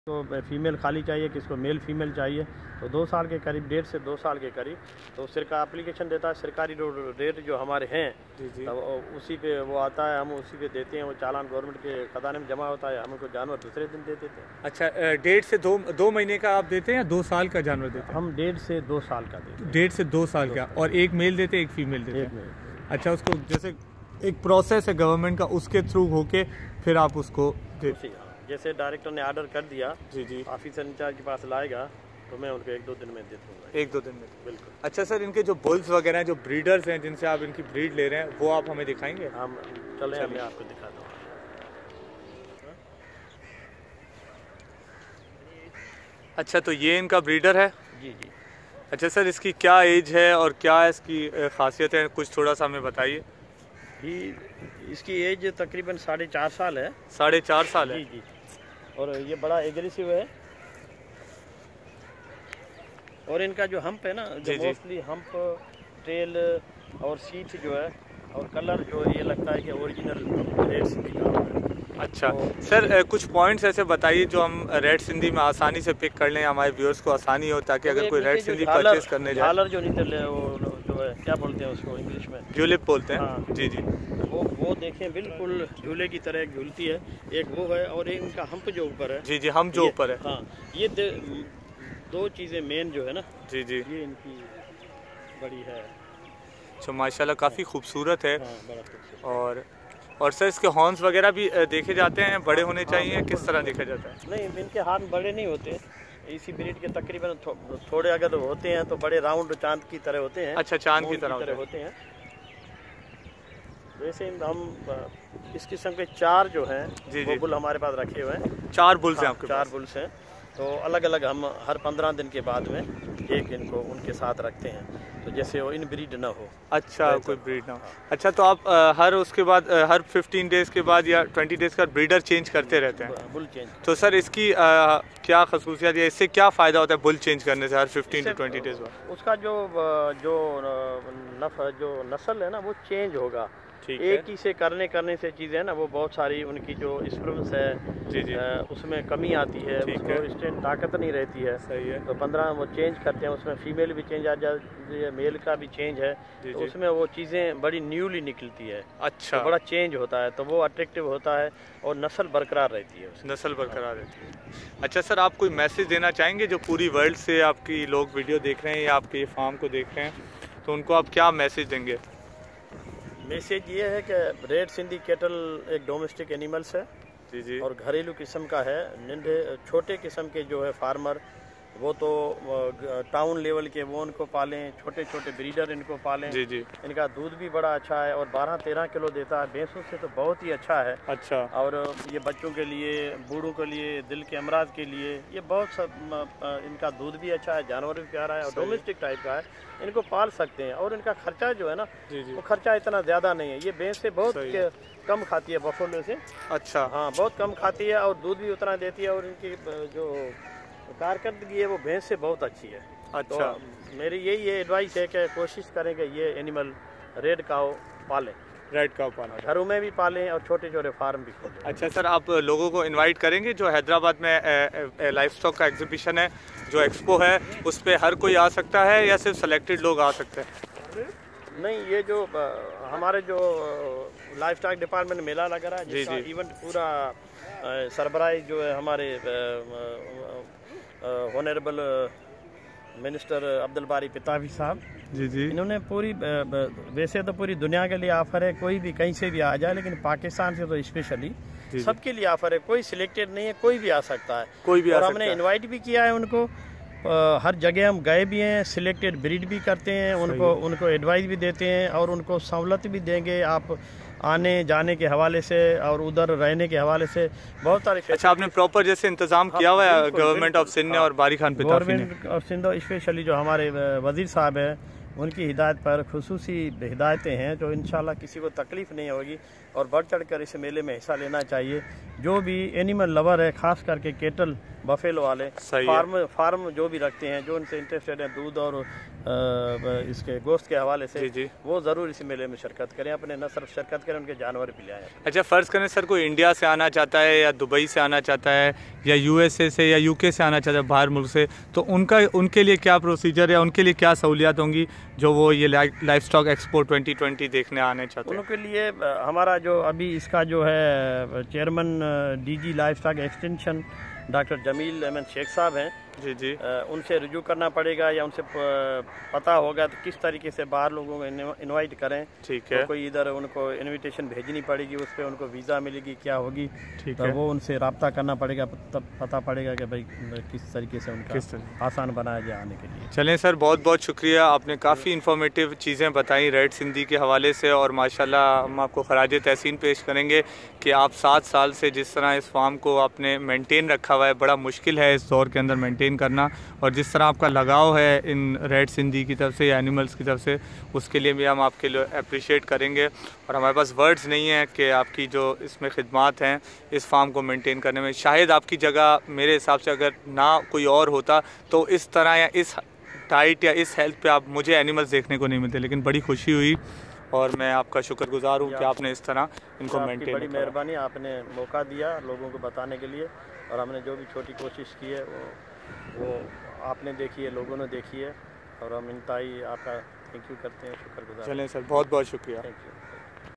How to Get Red Sindhi Cows from Government Farm| Documentary
How to Get Red Sindhi Cows from Government Farm located in Karachi. Complete Documentary